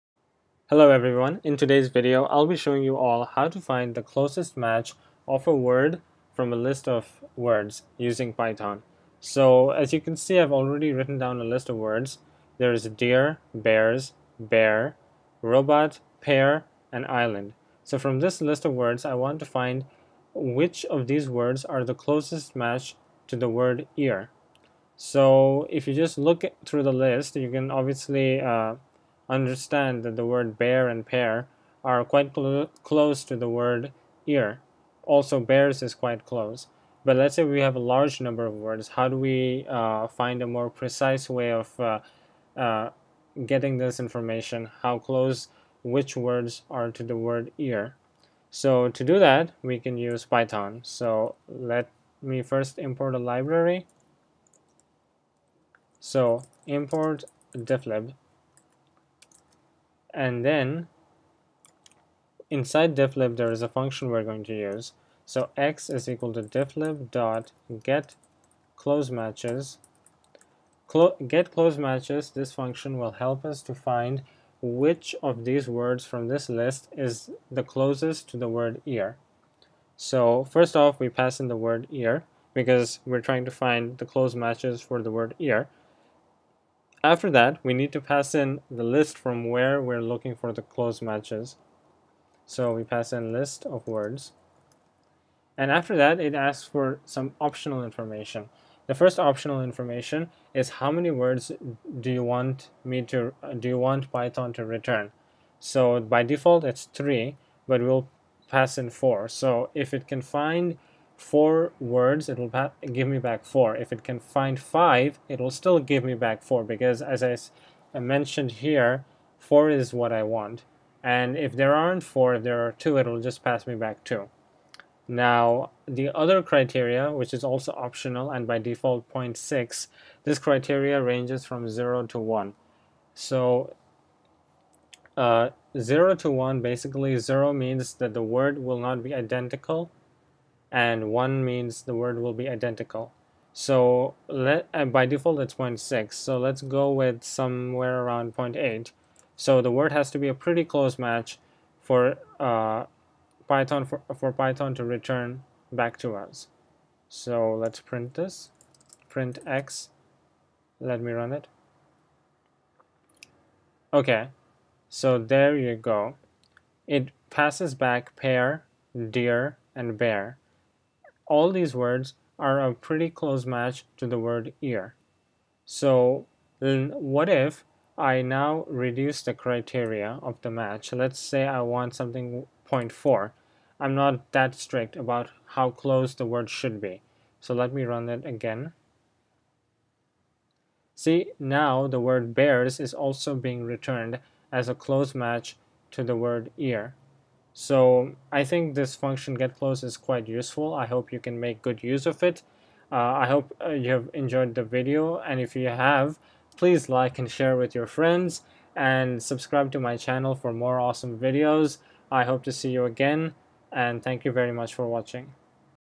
Get closest matches of a word using Python
The video explains in detail on how to use Python to find the closest matches of a word from a list of words.